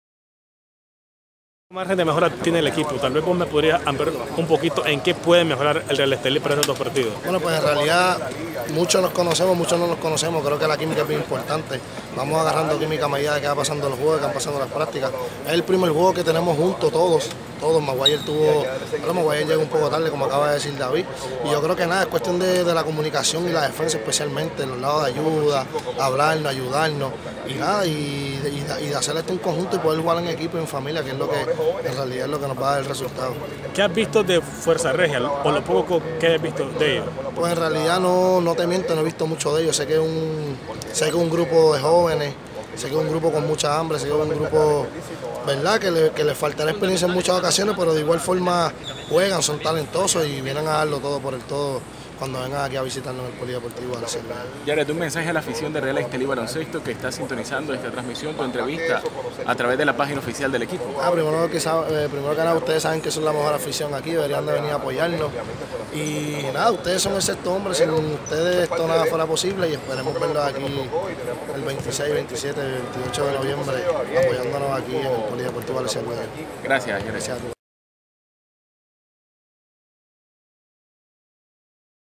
El plan del Real Estelí para mantener nivel en la BCL Americas
El Real Estelí aumentará la intensidad de los entrenamientos para llegar en mejor forma ante Fuerza Regia y Capitanes de México el 25 y 28 de noviembre en el Polideportivo Alexis Argüello